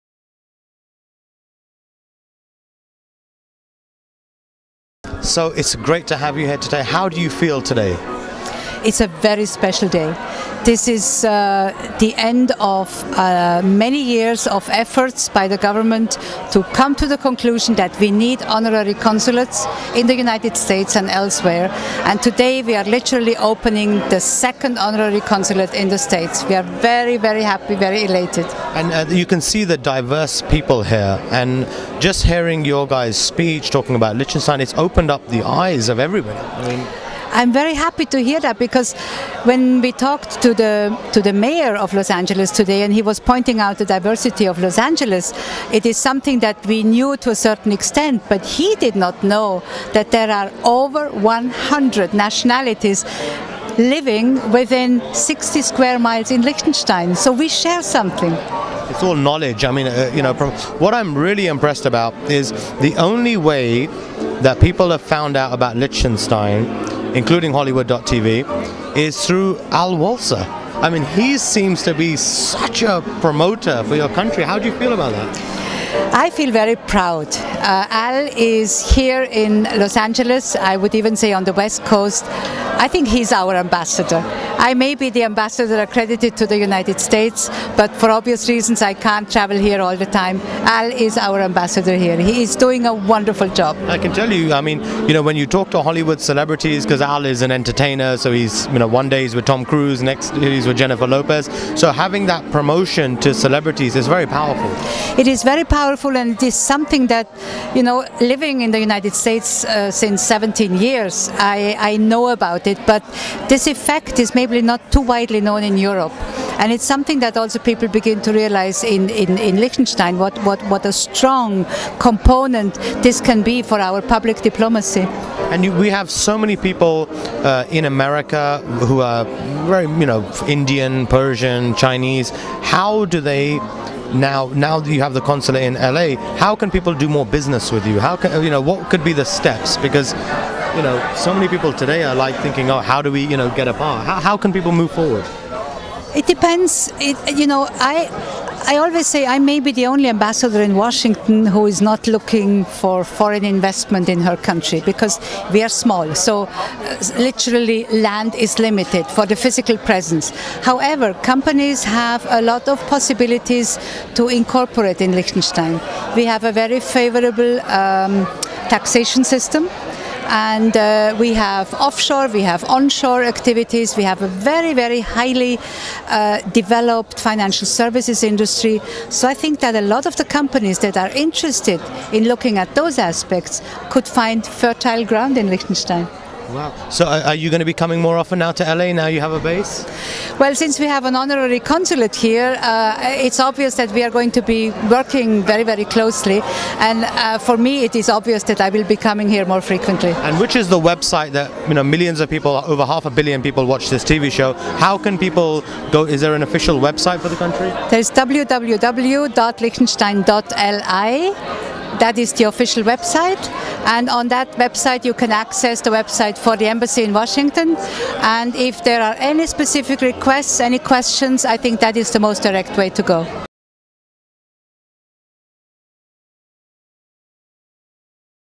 The Honorable Claudia Fritsche - Liechtenstein
Hollywood.TV interviews The Honorable Claudia Fritsche - Liechtenstein hosted by Al Walser